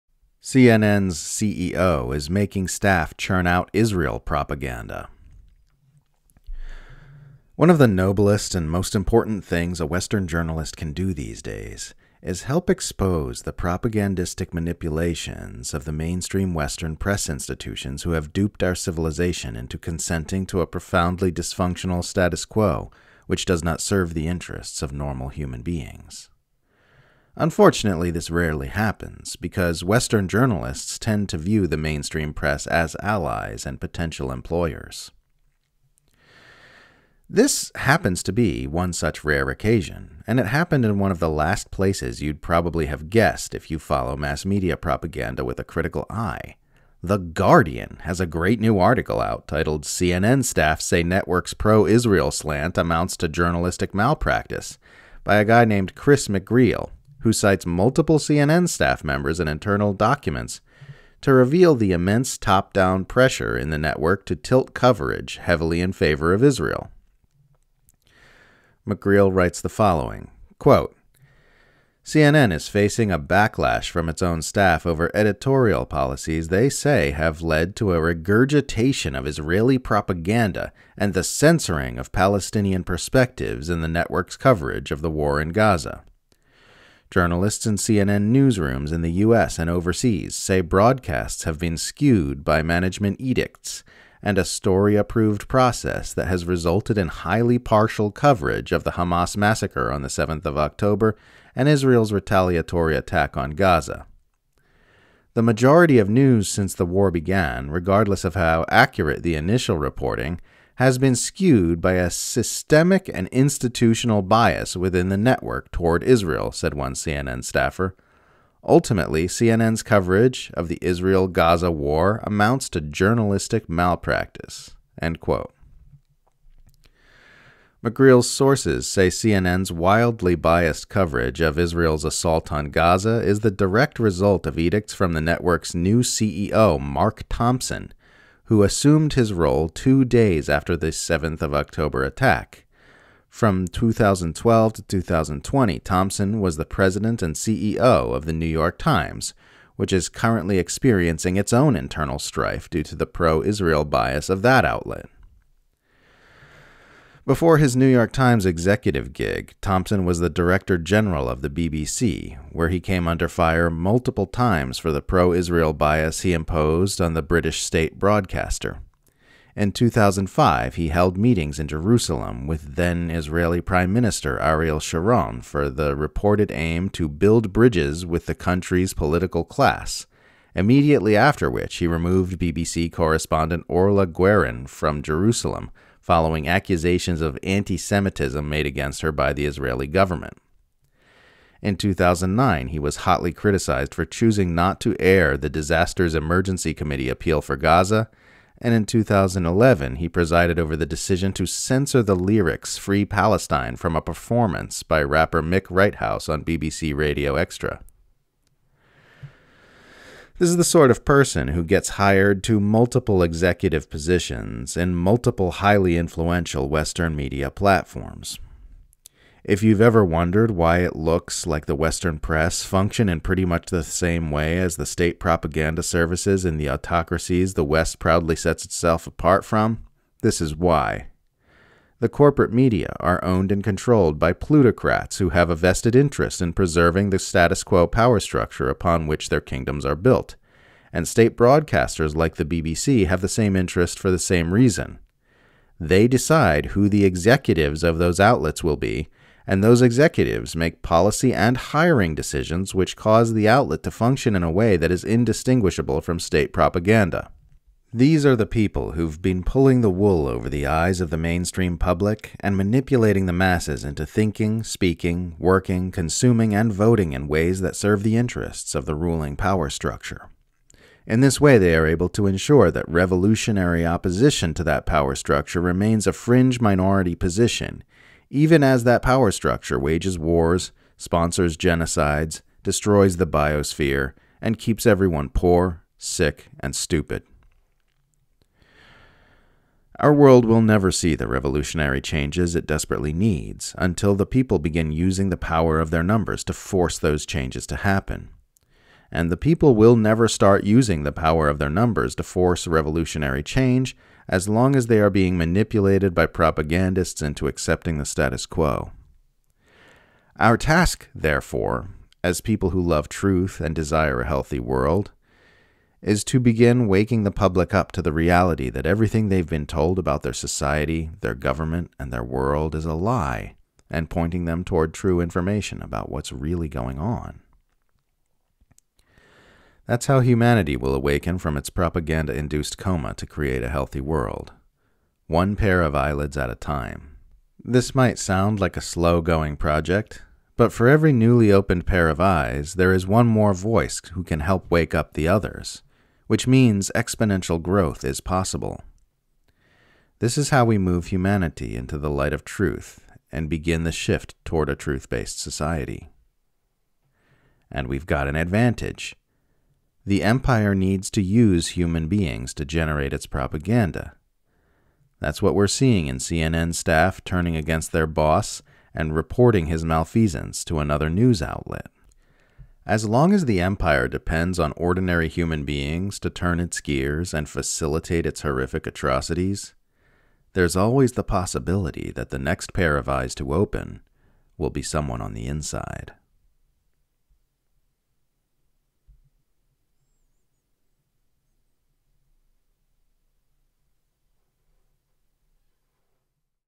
CNN's CEO Is Making Staff Churn Out Israel Propaganda
This is the sort of person who gets hired to multiple executive positions in multiple highly influential western media platforms. If you've ever wondered why it looks like the western press function in pretty much the same way as the state propaganda services in the autocracies the west proudly sets itself apart from, this is why. The corporate media are owned and controlled by plutocrats who have a vested interest in preserving the status quo power structure upon which their kingdoms are built, and state broadcasters like the BBC have the same interest for the same reason. They decide who the executives of those outlets will be, and those executives make policy and hiring decisions which cause the outlet to function in a way that is indistinguishable from state propaganda.